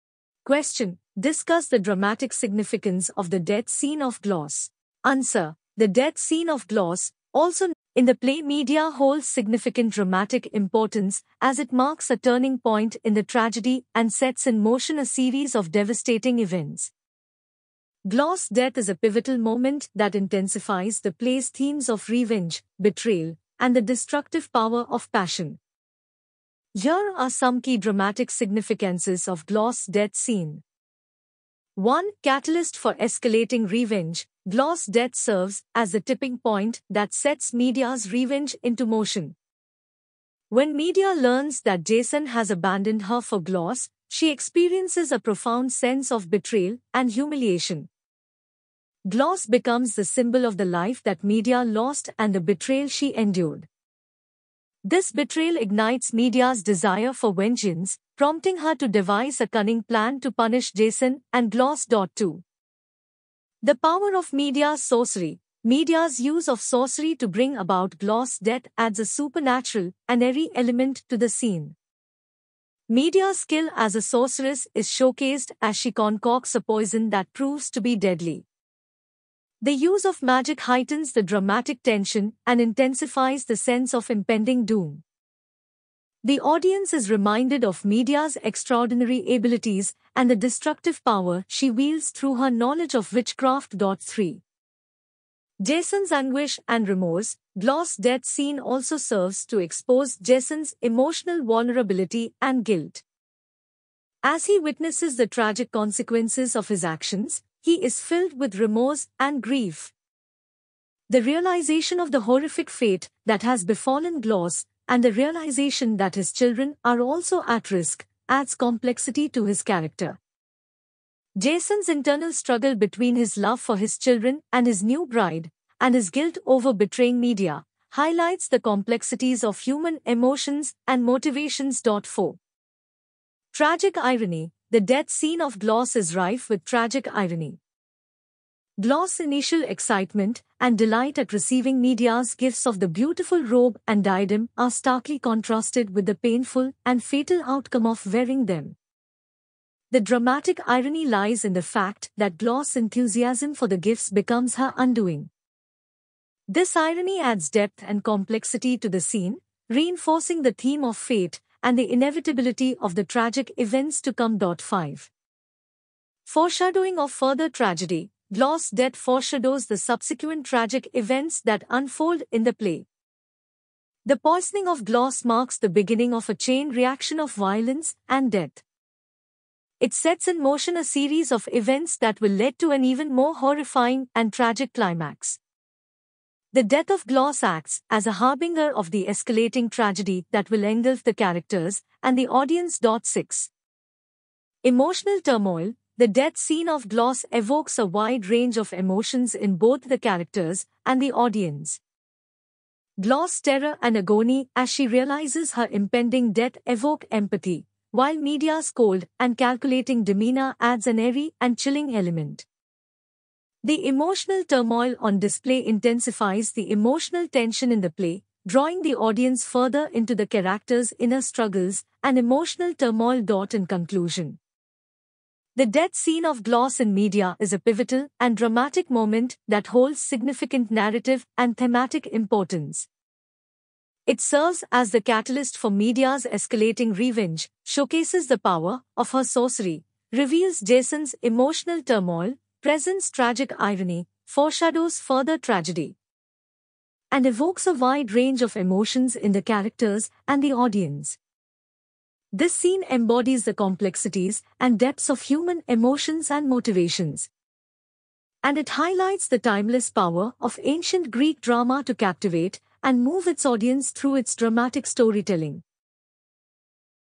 Medea: Dramatic significance of the death scene of Glauce.
Title: "Medea's Vengeance Unleashed: Unraveling the Dramatic Significance of Glauce's Death Scene"

Description: 🔥🎭

In this gripping video, we delve into the powerful world of Greek tragedy as we explore the dramatic significance of Glauce's death scene in the classic play, "Medea." Join us on a journey through the heart-wrenching tale of love, betrayal, and revenge, as we analyze the pivotal moment when Medea's thirst for vengeance reaches its climax. Discover the psychological depths of the characters, the intricate web of emotions, and the tragic consequences that follow. With insights and analysis, we aim to shed light on the timeless themes presented in this ancient masterpiece. Don't miss this captivating exploration of Medea's wrath and its impact on the tragic fate of Glauce.